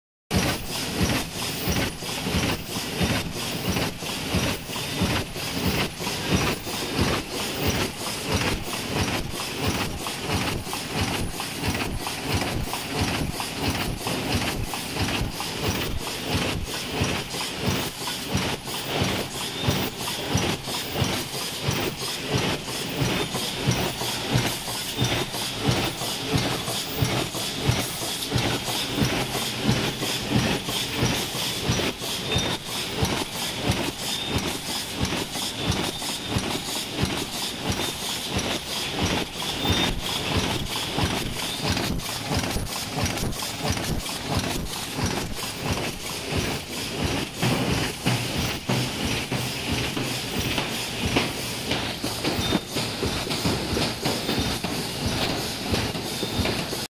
High speed paper cup machine paper bowl machine for salad bowl and take away container
CM200 paper bowl forming machine is designed to produce paper bowls with stable production speed up to 80-120pcs/min. For details, please contact

Available paper：
170-350gsm, Single and double PE / PLA / Aluminum or water-based barrier coated paper board

Competitive Advantage:
*The mechanical transmission is mainly by gears and CAM, with fully auto gear oil lubrication system, which guarantees machine stability, efficiency and longer service life.
*Folding wings, knurling wheel and brim rolling stations are adjustable above main table, no adjustment needed inside the main frame so that the work is much easier and time saving.
*Heaters are using Leister, which is well-known brand made in Swiss, ultrasonic for side seam supplemental.
*All motors are independent controlled by frequency invertors, these can adapt wider range of paper character.